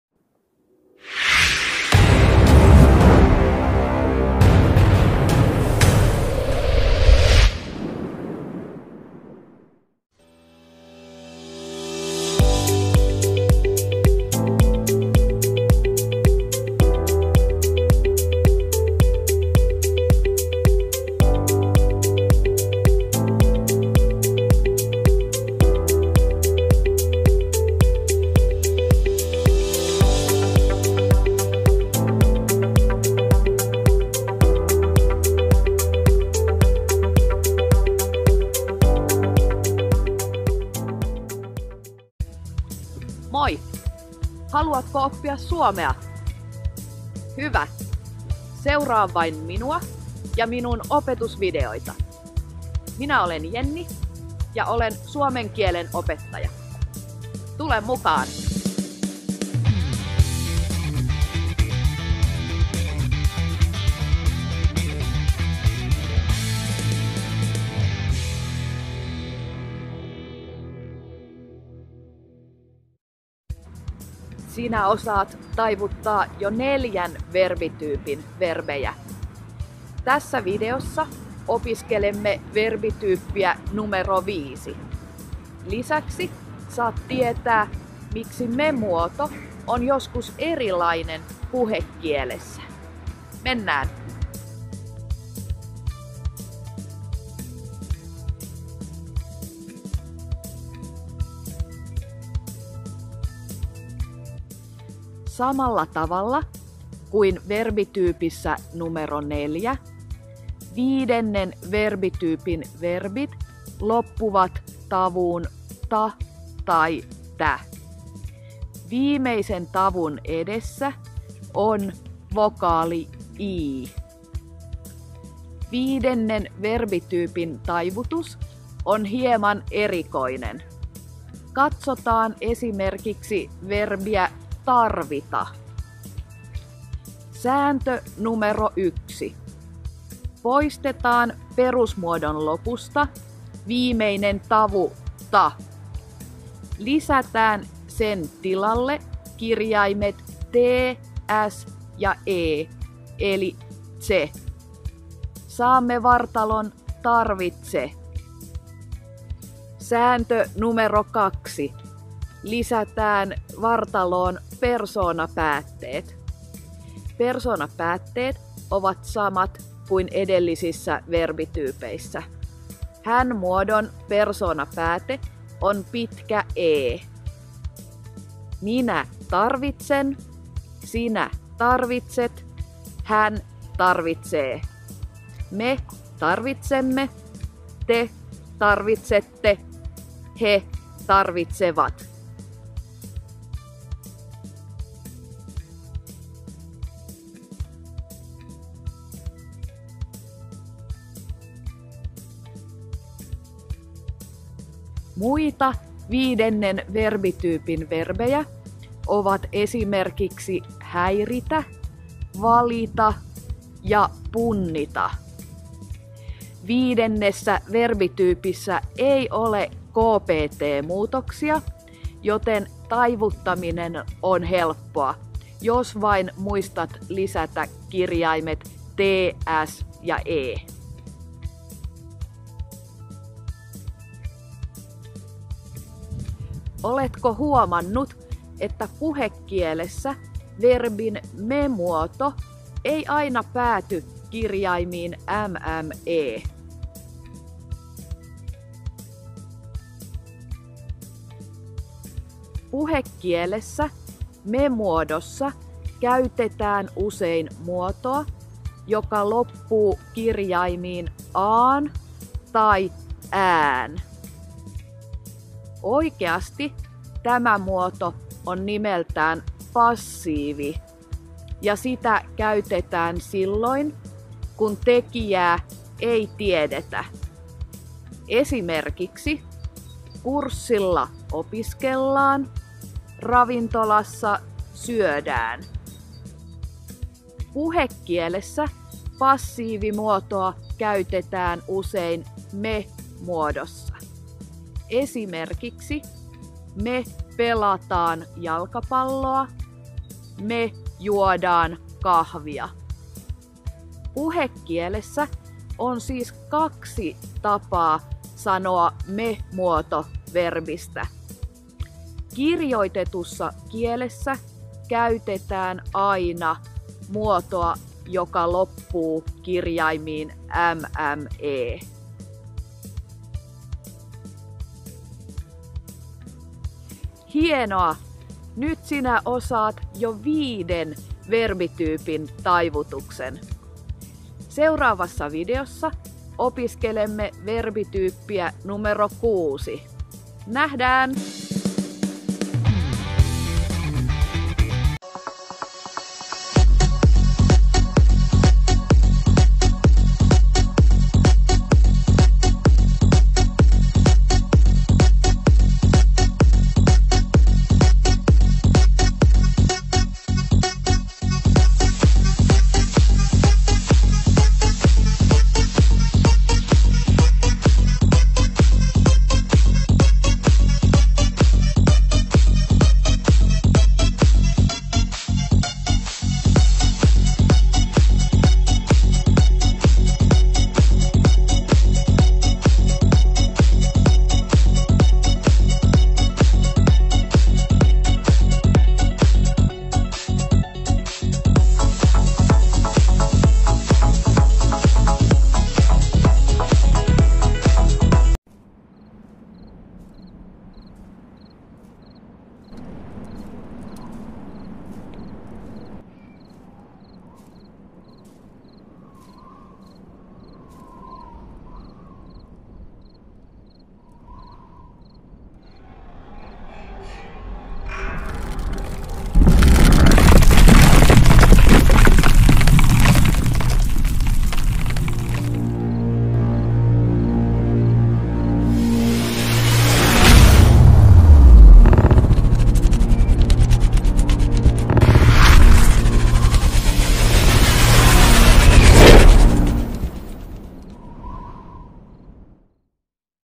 تعلم تصريف الافعال في اللغة الفنلندية الخامسة مع المعلمة Jenni / suomen verbit osa 5
Mahmoud_Kaddah/opi suomea helposti